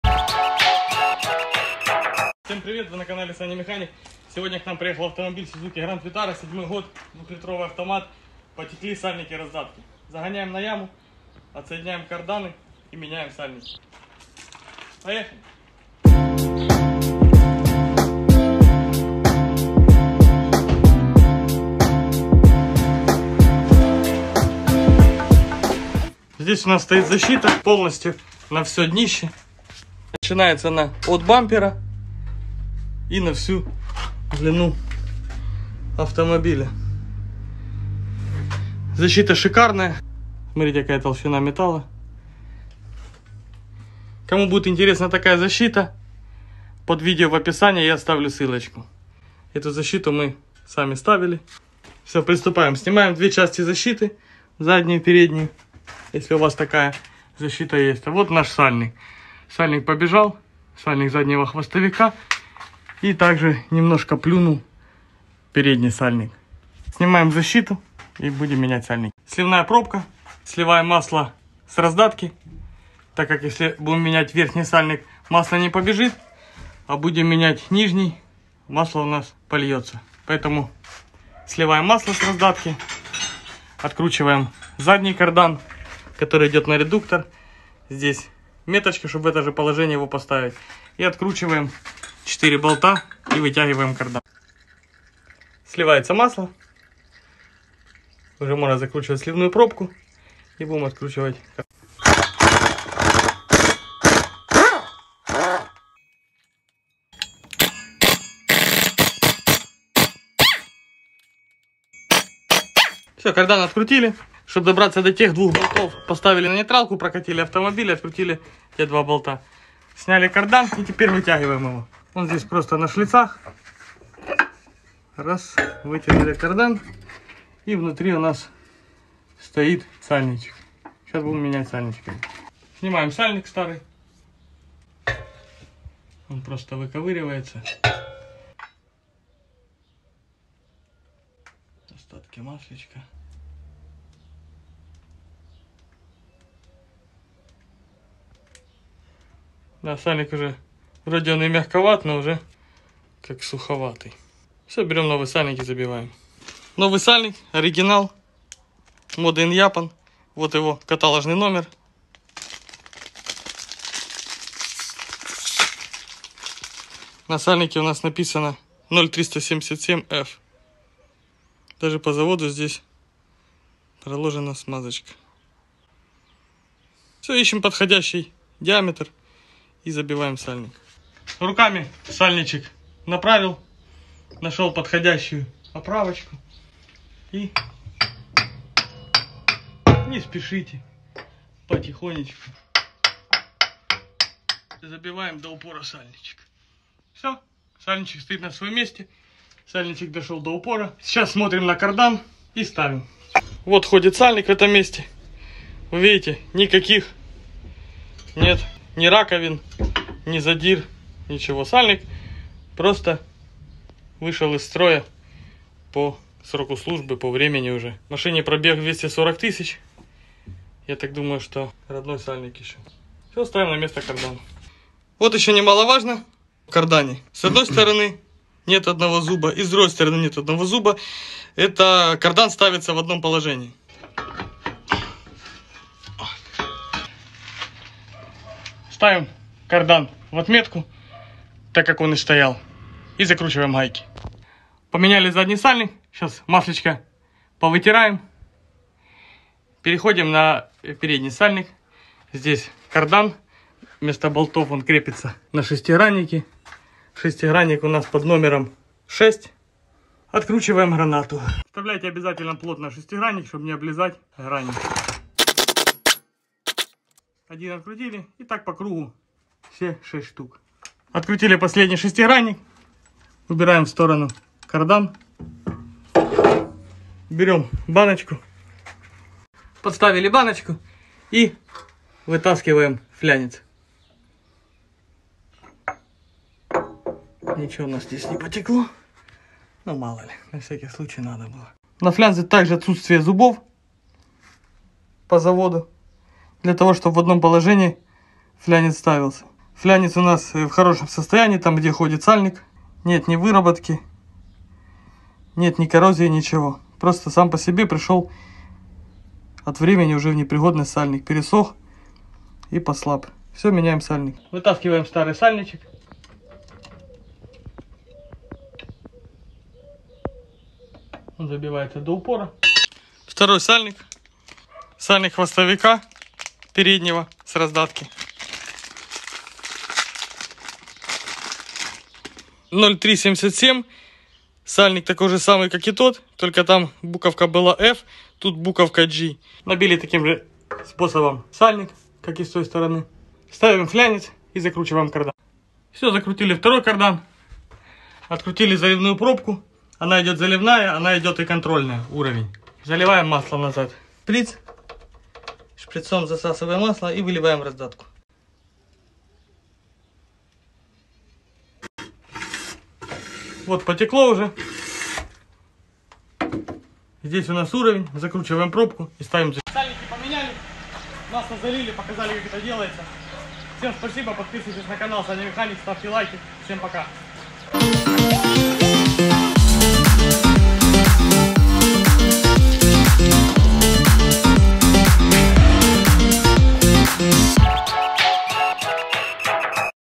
Замена сальников раздатки Сузуки Гранд Витара 2 / Как менять сальники раздатки Suzuki Grand Vitara 2
Сегодня на ремонте Сузуки Гранд Витара 2 поколение 2007 года выпуска 2 литра, автомат, в видео покажем как менять сальники раздатки на Suzuki Grand Vitara 2. Поехали!

Задавайте вопросы в комментариях под видео!